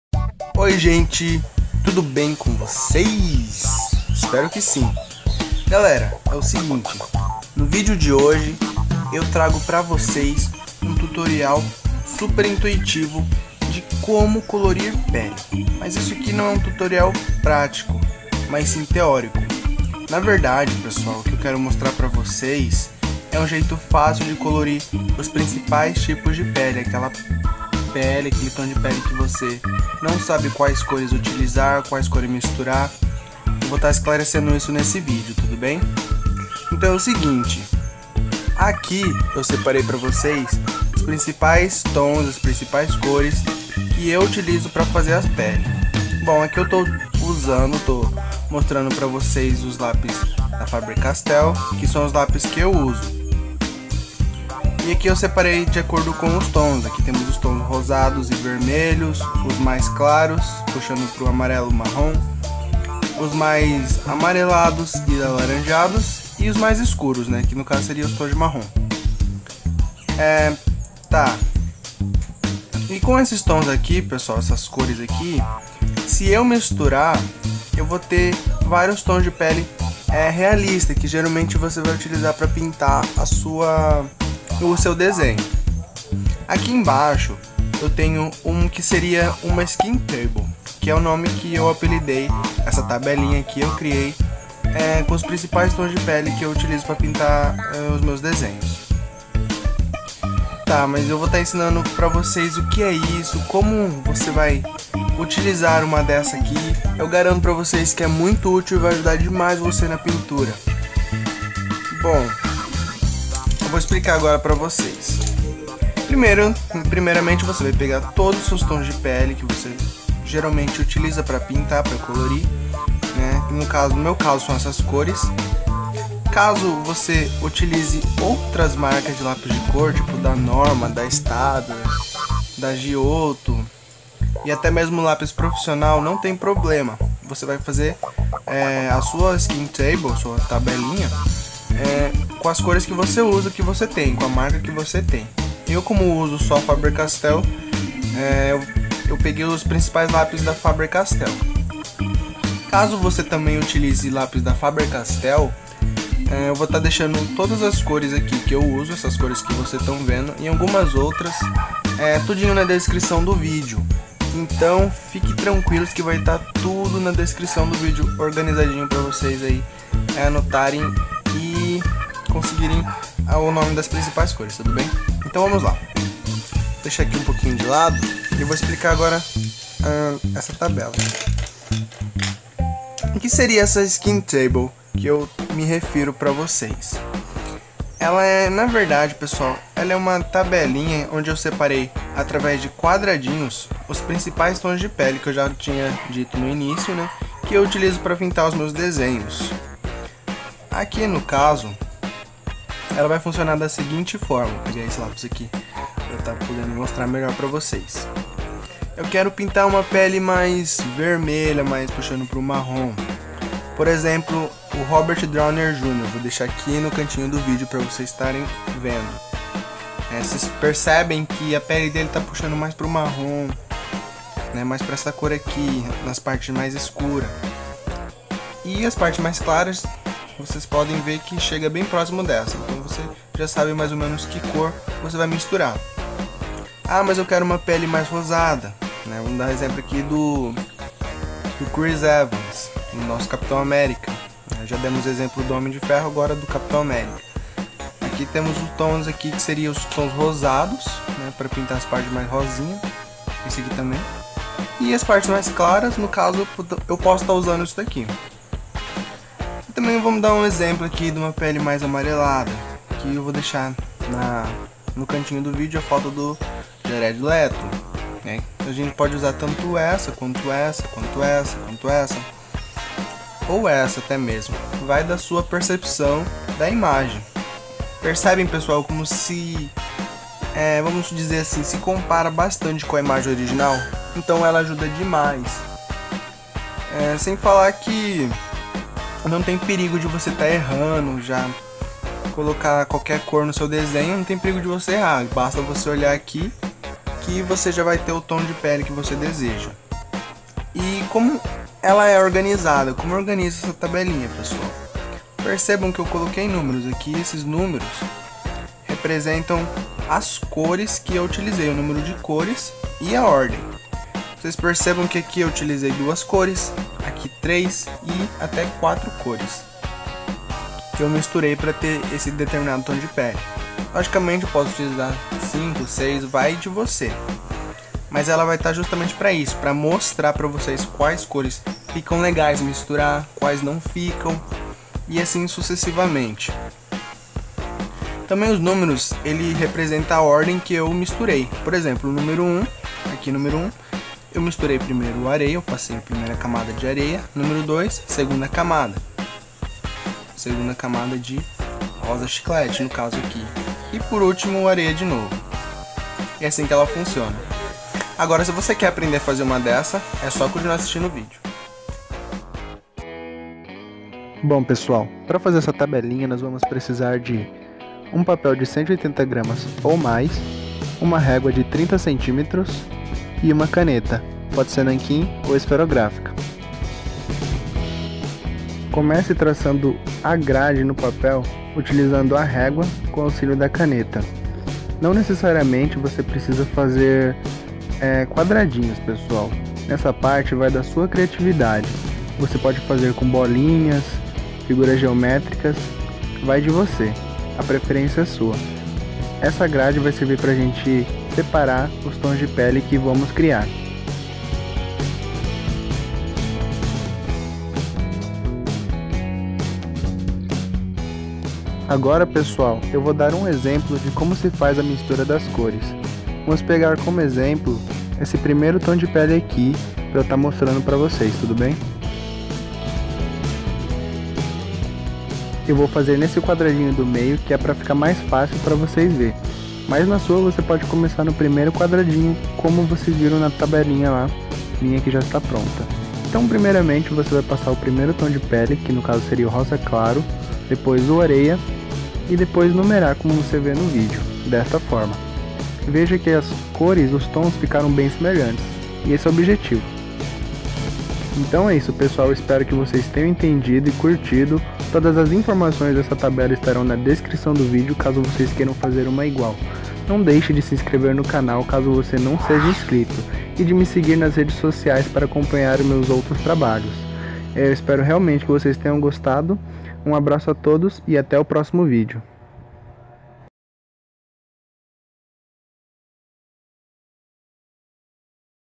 Aprenda a colorir os principais tons de pele! (How to color skin)
Skin tones! these are the colors that I use coloring skin;
Tons de Pele! Essas são as cores que eu uso para colorir a pele;

                        CORES/ COLORS

1) Light pink, salmon, bubblegum pink and Petal rose

2) sand and ocher burned

3) light orange, ocher Orange, light brown and Ocher.

4) bronze, cinnamon, burgundy, burnt umber, Brown, dark Brown, and cocoa.

1) Rosa claro, salmom, rosa chiclete e rosa petala

2) areia e ocre queimado

3) laranja claro, ocre alaranjado, marrom claro e ocre

4) bronze, canela, chocolate, marrom, marrom escuro, bordô e sépia.

All colors are Faber Castell/ Todas as cores são da Faber Castell.